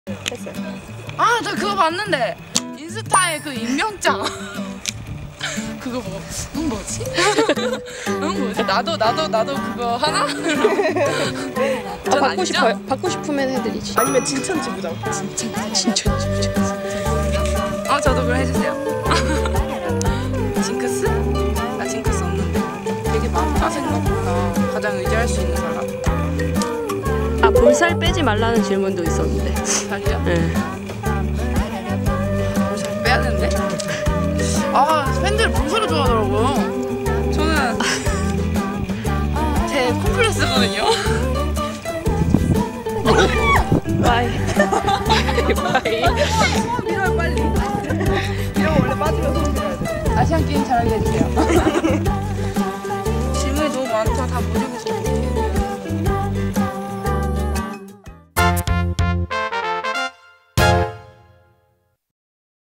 [코보티비] "내 소듕한 종이인데~(울컥)"
으앙 그거 내 소듕한 질문 종이인데~ ㅠㅠ
팬들은 좋아하지만 김채연 선수는 콤플렉스로 생각하는 그것은?